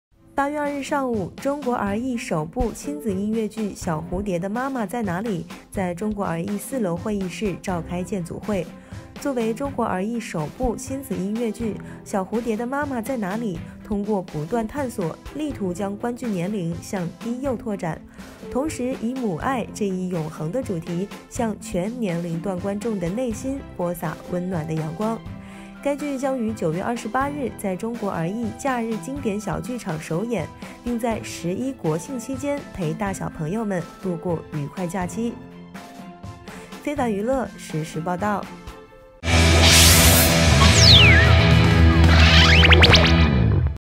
【1年前】中國兒藝親子兒童劇《小蝴蝶的媽媽在哪裡？》于8月2日建組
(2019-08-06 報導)

8月2日上午，中國兒藝首部親子音樂劇《小蝴蝶的媽媽在哪裡？》在中國兒藝四樓會議室召開建組會。中國兒藝院長、黨委副書記尹曉東，黨委書記、副院長馮俐，副院長閃增宏，黨委副書記兼副院長楊帆，副院長趙寒冰、于筱燕和劇組主創、演員及相關部門負責人參加了此次建組會。作為中國兒藝首部親子音樂劇，《小蝴蝶的媽媽在哪裡？》通過不斷探索，力圖將觀劇年齡向低幼拓展，同時以母愛這一永恆的主題，向全年齡段觀眾的內心播撒溫暖的陽光。該劇將于9月28日在中國兒藝假日經典小劇場首演，并在“十·一”國慶節期間陪大小朋友們度過愉快假期。